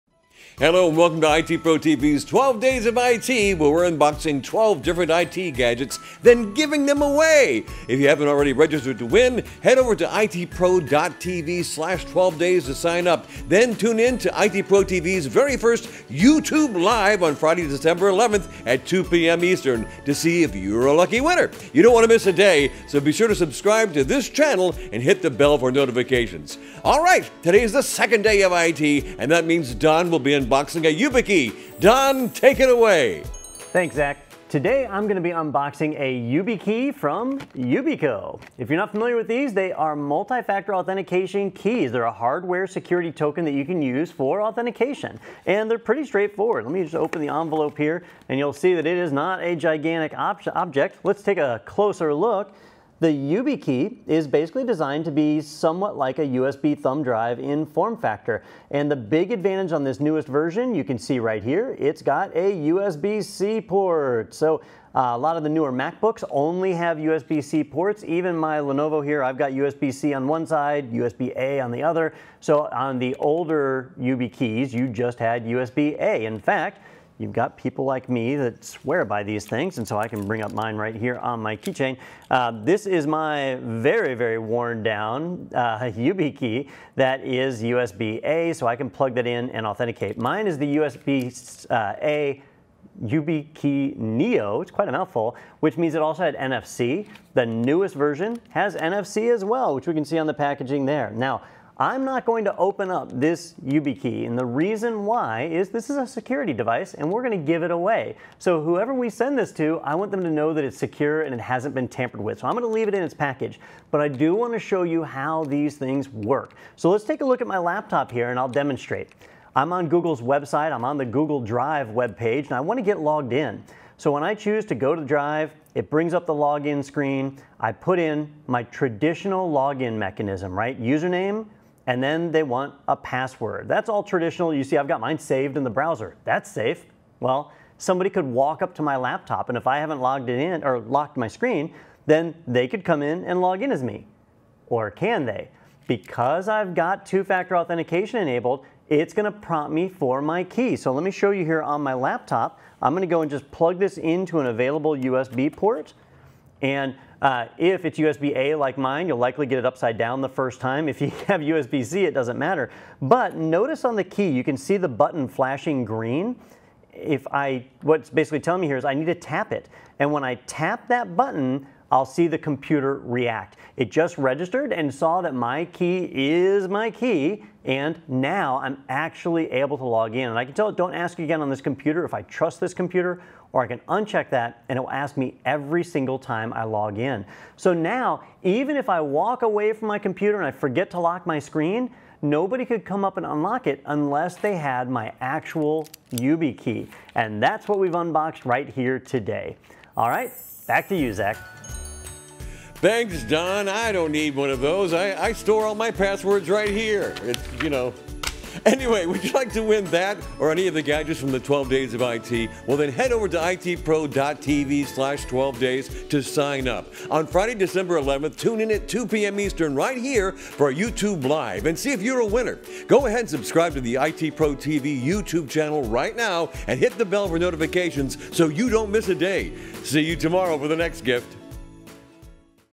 Day 2 of 12 Days of IT Giveaway | Yubico Yubikey 5C NFC Unboxing
It's day two of the 12 Days of IT, so Don is unboxing the brand new Yubikey 5c NFC from Yubico. Unboxing is pretty simple on this one since it's just one piece, but Don takes the time to show a demo of how the Yubikey can keep you secure with multi-factor authentication.

Want to win this device or any of the other items from the 12 Days of IT? now to enter and join us live on December 11th at 2pm US (EST) to see if you won